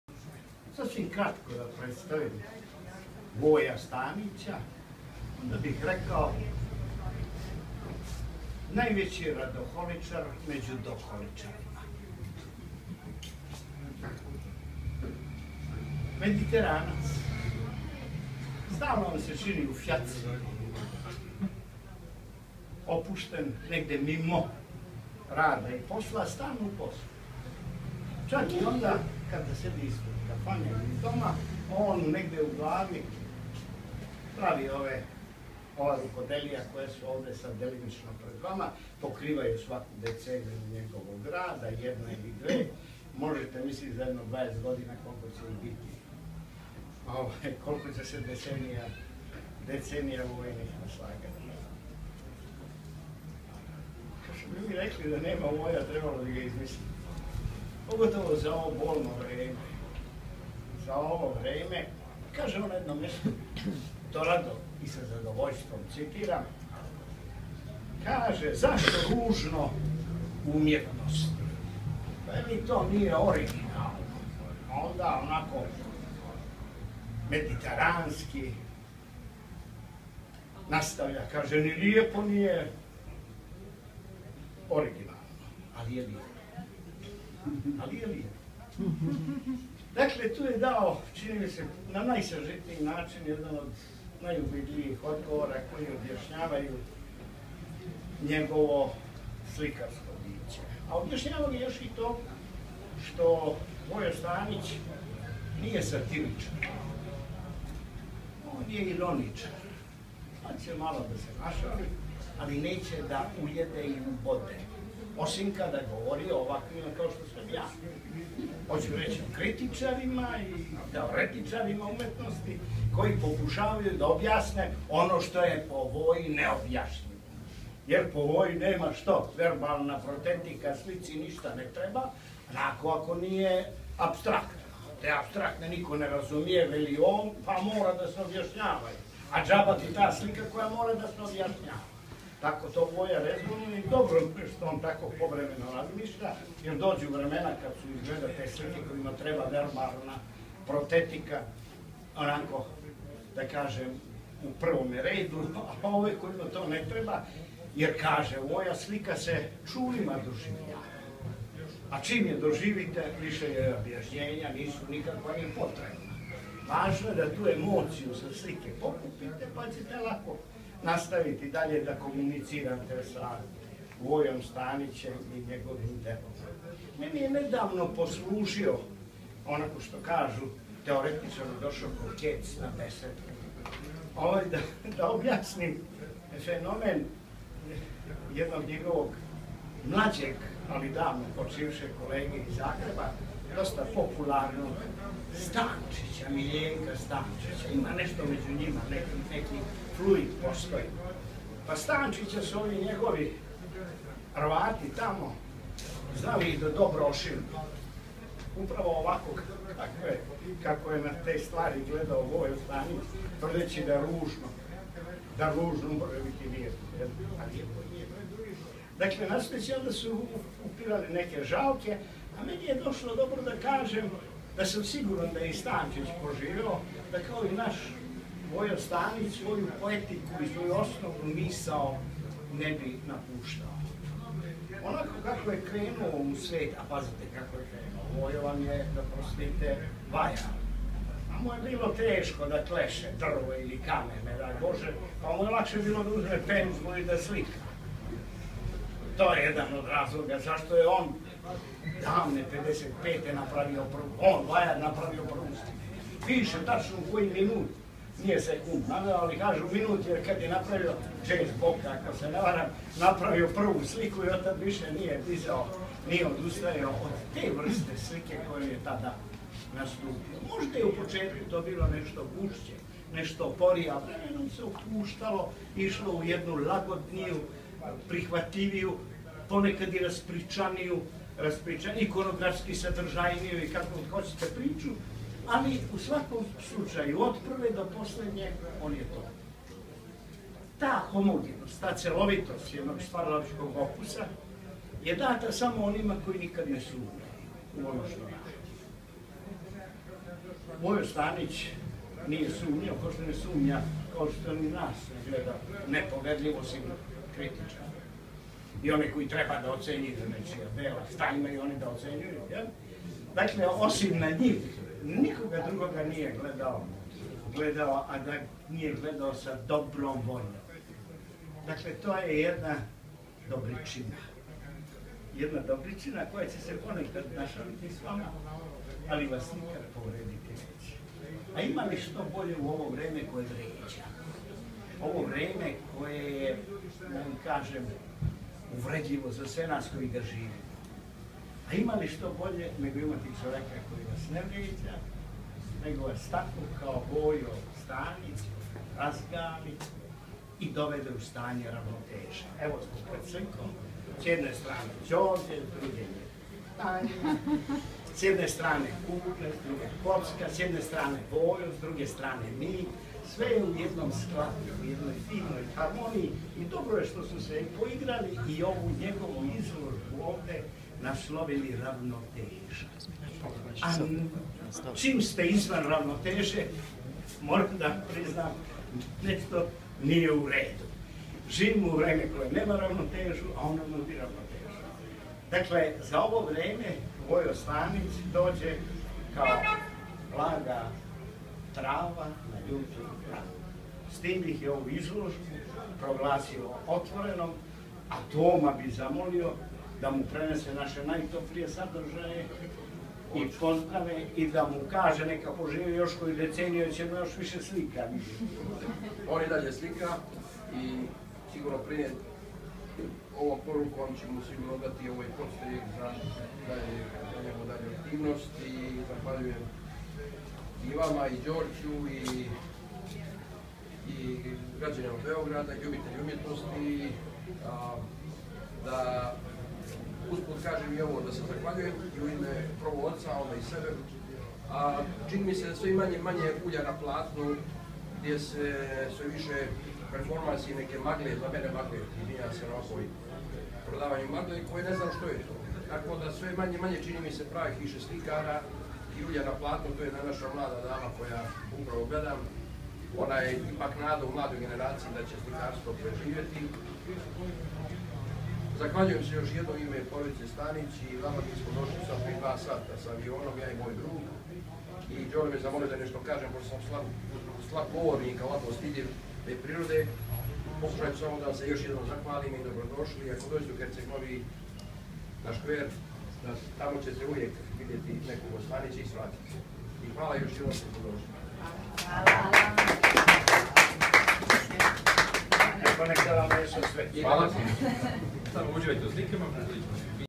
Otvaranje izlozbe slika Voja Stanica, Art Gallery Canvas, 30.09.2014
Izlozbu otvorio Nikola Kusovac. Preneo pozdrav od Voja Stanica i pozdravio goste Tomislav Stanic